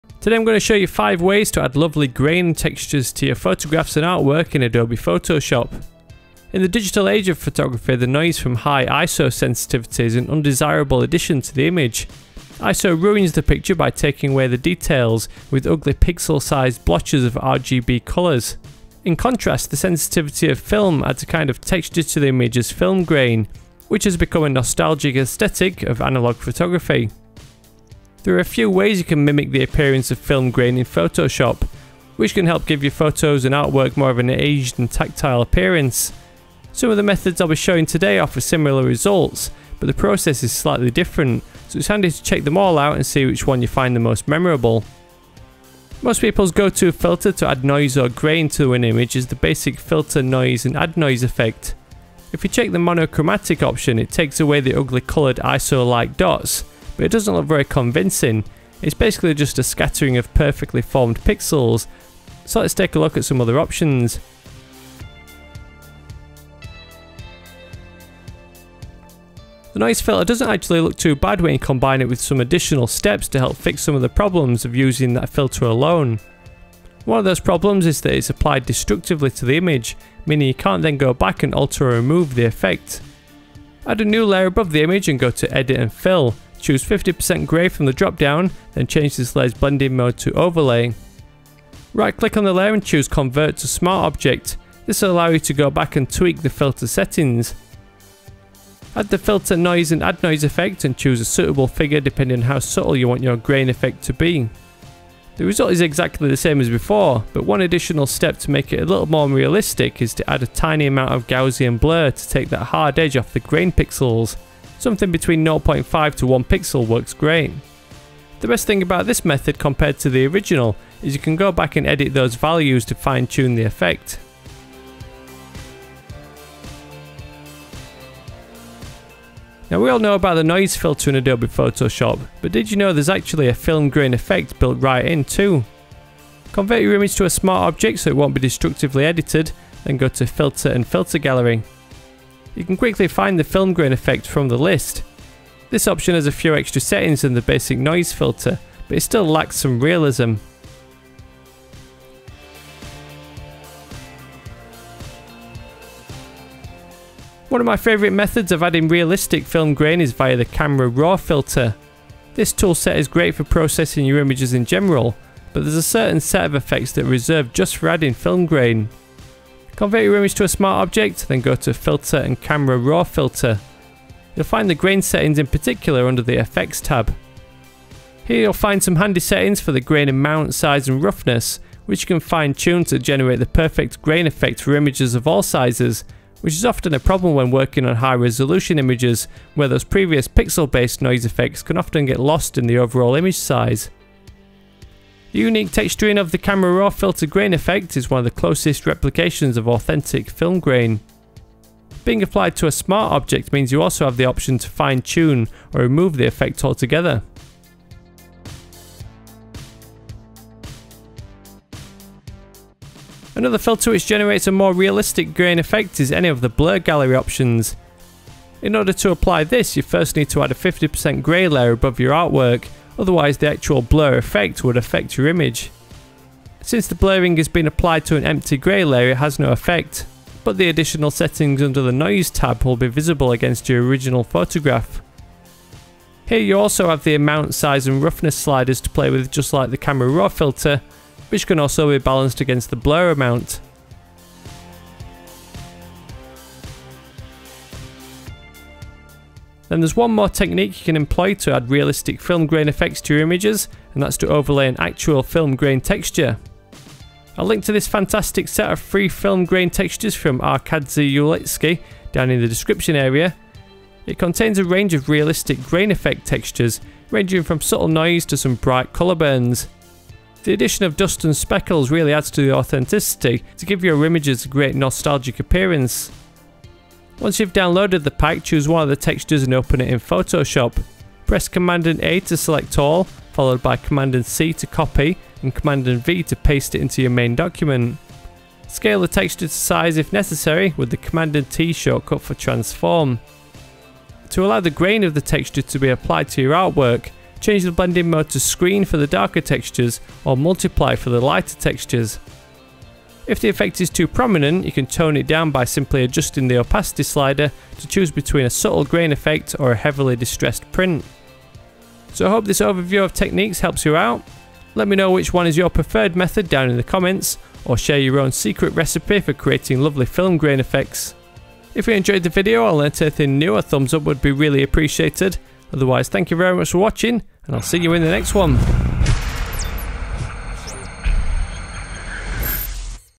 5 Ways To Create Film Grain Effects in Adobe Photoshop
Today I'm going to show you 5 ways to add lovey grain textures to your photographs and artwork in Adobe Photoshop. In the digital age of photography the noise from high ISO sensitivity is an undesirable addition to the image. ISO ruins the picture by taking away the details with ugly pixel sized blotches of RGB colours. In contrast, the sensitivity of film adds a kind of texture to the image as film grain, which has become a nostalgic aesthetic of analogue photography.
There are a few ways you can mimic the appearance of film grain in Adobe Photoshop, which can help give your photos and artwork more of an aged and tactile appearance. Some of the methods I'll be showing offer similar results, but the process is slightly different, so it's handy to check them all out and see which one you find most memorable.